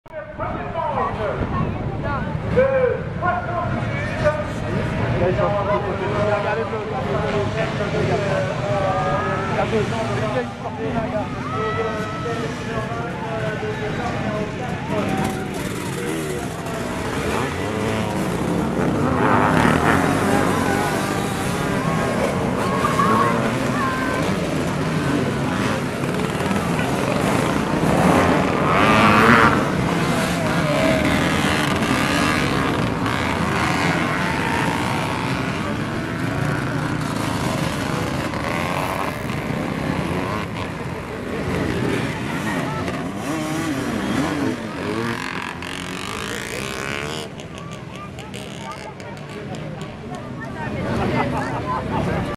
La Chinelle 2014 1er départ ancienne, ancètre.
1er départ en ancienne/ancêtre aux 2 H de la Chinelle 2014.
Un 500 HVA XC en tête  après le 2ème virage !